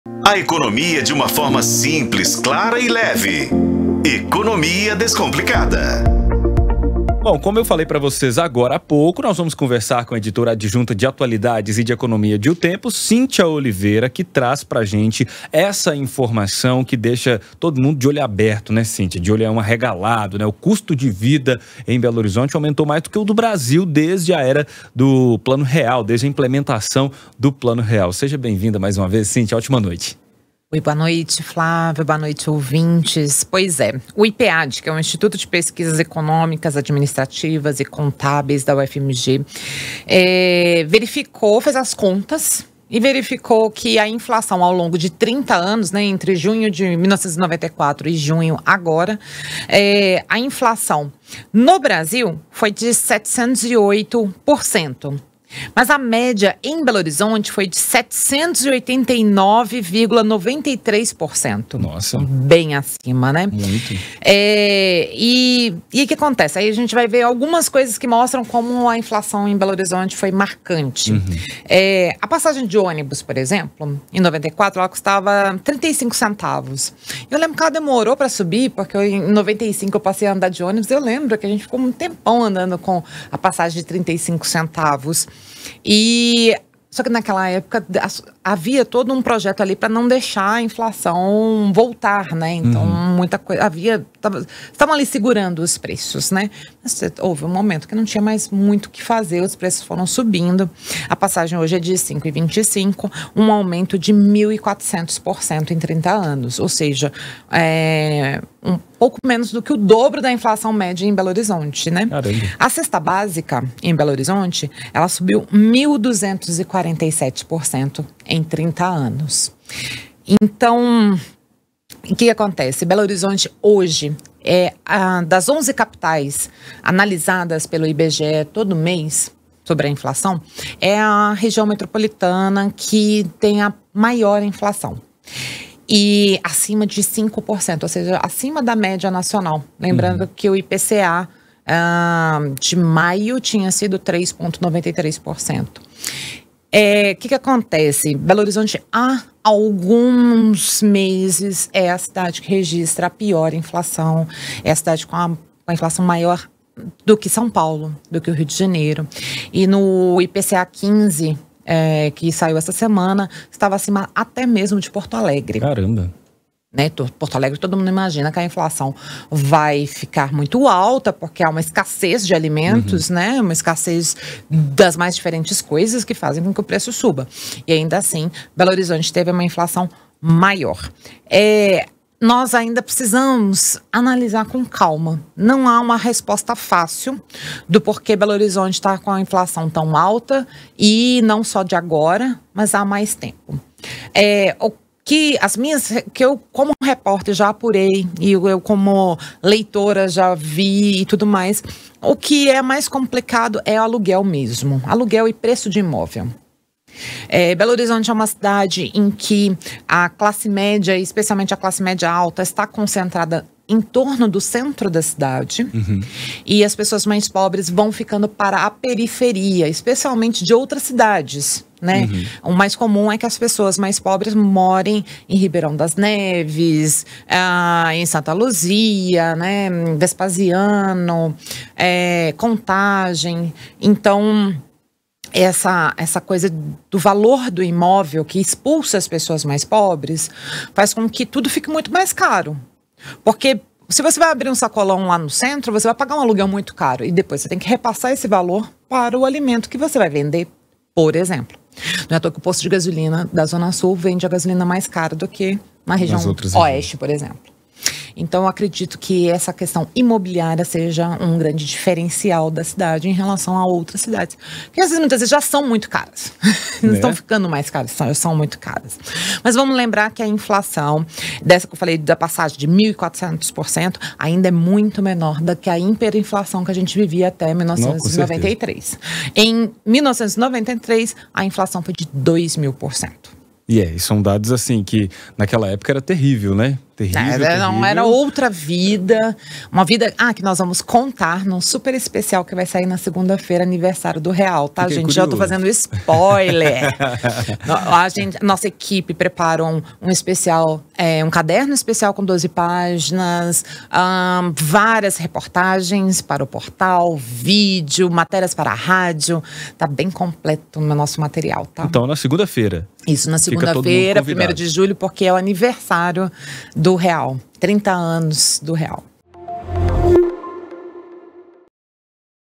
Custo de vida em BH aumentou mais do que no Brasil desde o Plano Real
Cinthya Oliveira comenta pesquisa do Ipead/UFMG que revela que a inflação, em BH, em 30 anos, foi de 789%.

00:00 DEIXA SEU LIKE!
02:00 SE INSCREVA NO CANAL